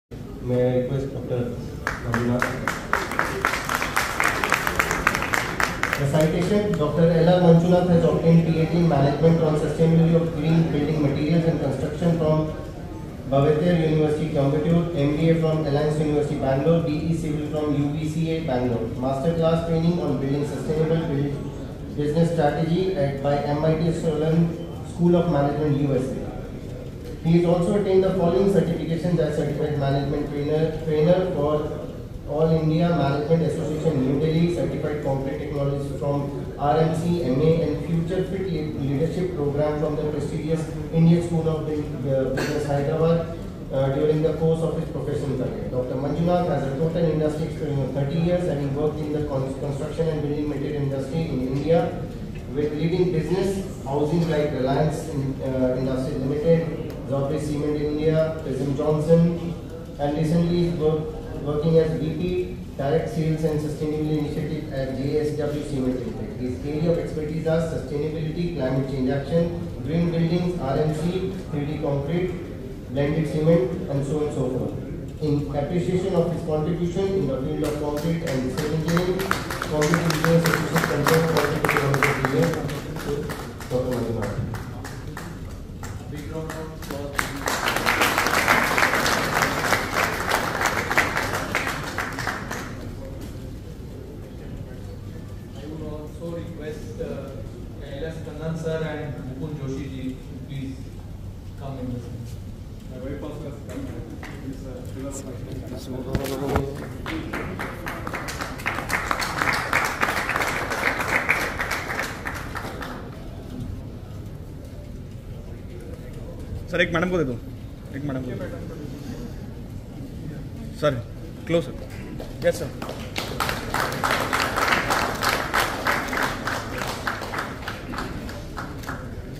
Concrete Technologist of India 2023 Award citation at New Delhi
Video from Dr.LRM